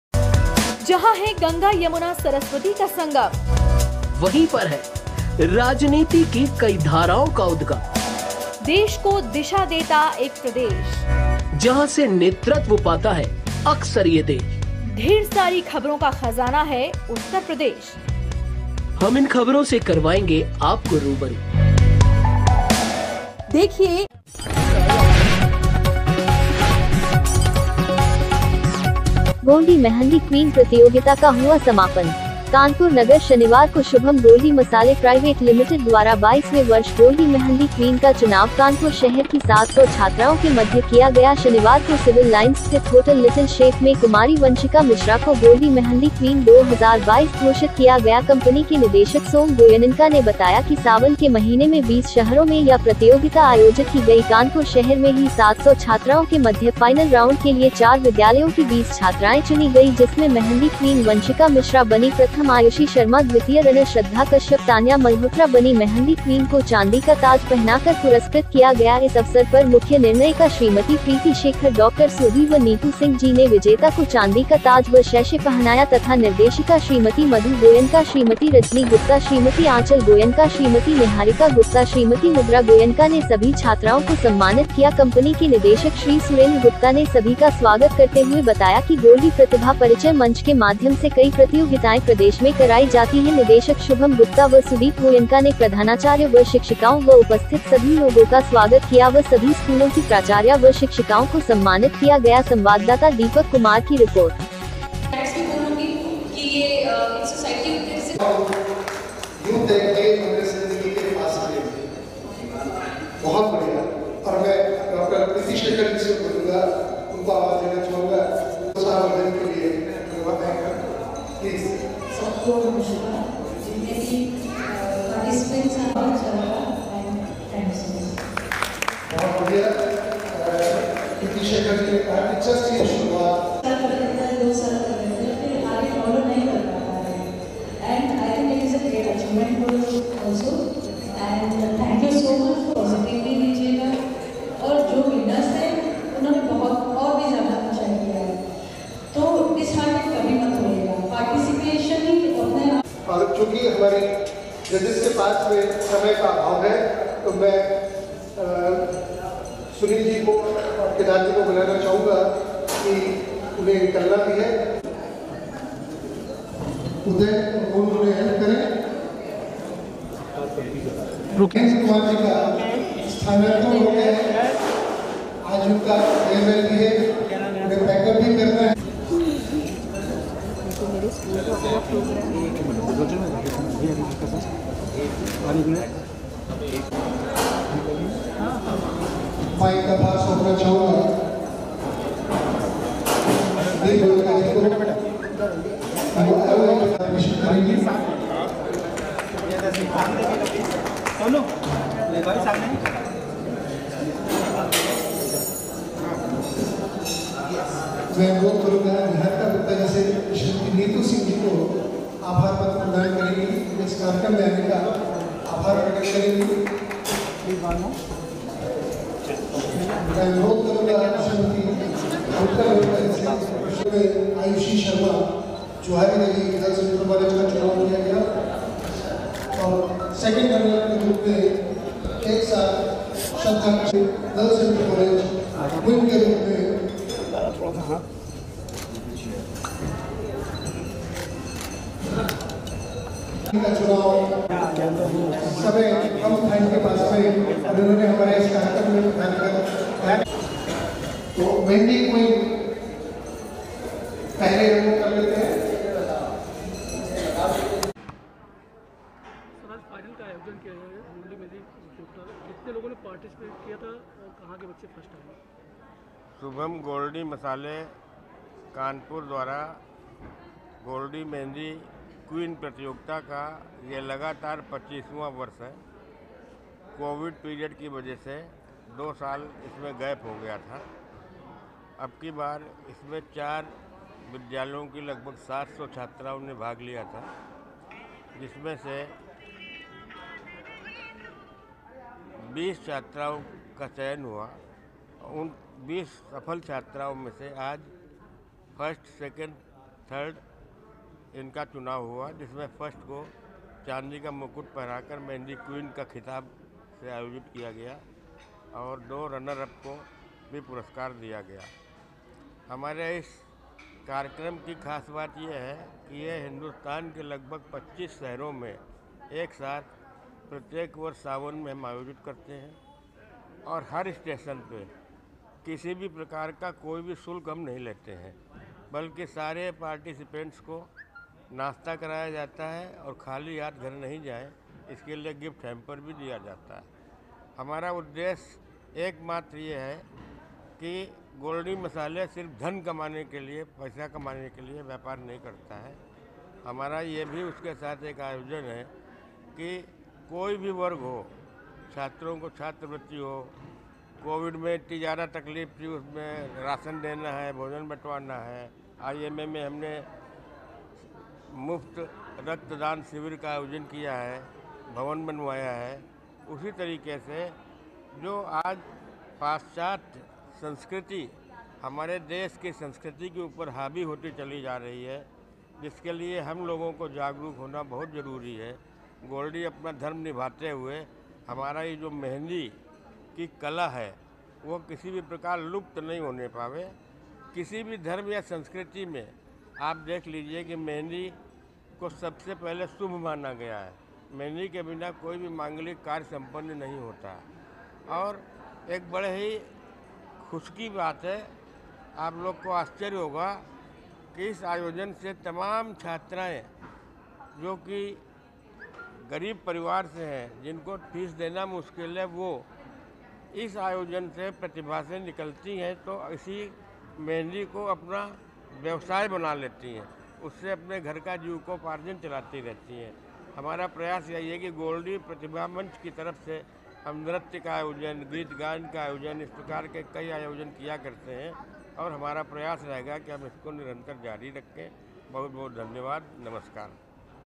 गोल्डी मेहंदी क्वीन प्रतियोगिता का हुआ समापन।।
कानपुर नगर- शनिवार को शुभम गोल्डी मसाले प्राइवेट लिमिटेड द्वारा 22वे वर्ष गोल्डी मेहंदी क्वीन का चुनाव कानपुर शहर की 700 छात्राओं के मध्य किया गया शनिवार को सिविल लाइंस स्थित होटल लिटिल शेफ मे कुमारी वंशिका मिश्रा को गोल्डी मेहंदी क्वीन 2022 घोषित किया गया कंपनी के निदेशक सोम गोयंनका ने बताया कि सावन के महीने में 20 शहरों में या प्रतियोगिता आयोजित की गई कानपुर शहर में ही 700 छात्राओं के मध्य फाइनल राउंड के लिए 4 विद्यालयों की 20 छात्राएं चुनी गई जिसमे मेहंदी क्वीन वंशिका मिश्रा बनी प्रथम आयुषी शर्मा द्वितीय रनर श्रद्धा कश्यप तान्या मल्होत्रा बनी मेहंदी क्वीन को चांदी का ताज पहनाकर पुरस्कृत किया गया इस अवसर पर मुख्य निर्णय का श्रीमती प्रीति शेखर डॉ सुरभि व नीतू सिंह जी ने विजेता को चांदी का ताज व शैशे पहनाया तथा निर्देशिका श्रीमती मधु गोयंका श्रीमती रजनी गुप्ता श्रीमती आंचल गोयंका श्रीमती निहारिका गुप्ता श्रीमती मुद्रा गोयनका ने सभी छात्राओं को सम्मानित किया कंपनी के निदेशक श्री सुरेंद्र गुप्ता ने सभी का स्वागत करते हुए बताया कि गोल्डी प्रतिभा परिचय मंच के माध्यम से कई प्रतियोगिताएं प्रदेश में कराई जाती हैं निदेशक शुभम गुप्ता व सुदीप गोयंका ने प्रधानाचार्य व शिक्षिकाओं व उपस्थित सभी लोगो का स्वागत किया व सभी स्कूलों की प्राचार्या व शिक्षिकाओं को सम्मानित किया गया